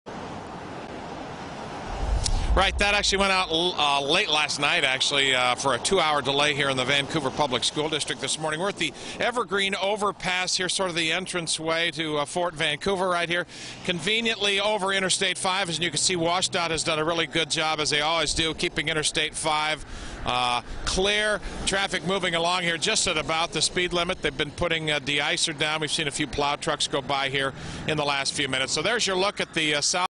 Overnight snow impacting morning commute in Vancouver
Traffic is moving pretty smoothly on Vancouver highways, but side roads are a bit messier after the snowfall.